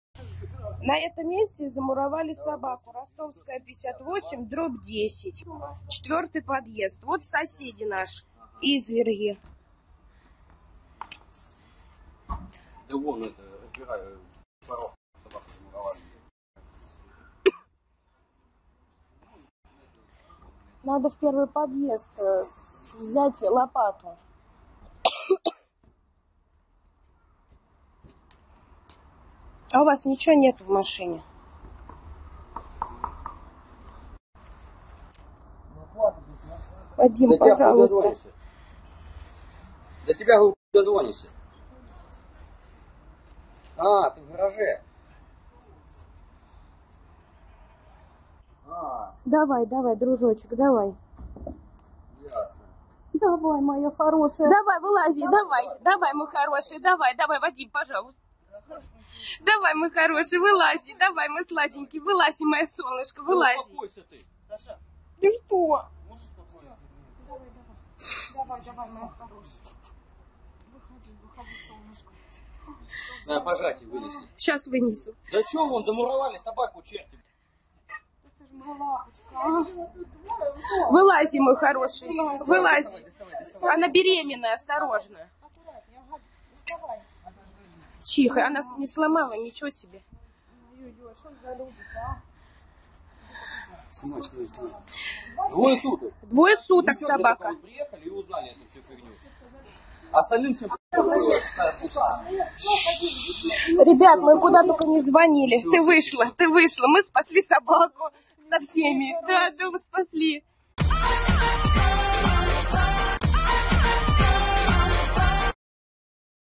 Pregnant Dog Gets Rescued After Being Trapped Beneath The Steps
He's A Hero: Man Rescues A Dog That Was Trapped Underground!
The dog was pregnant and had a den via a hole that lead under the stairs. Workers filled the hole without realizing the dog was there. Owners went looking for the missing dog and heard whimpering under the stairs. Workers refused to come back to free the dog, so some dude chiseled her out himself.

 Homeowners heard the muffled barking of a dog that was coming from underneath the ground! Workers unknowingly bricked up the front porch steps.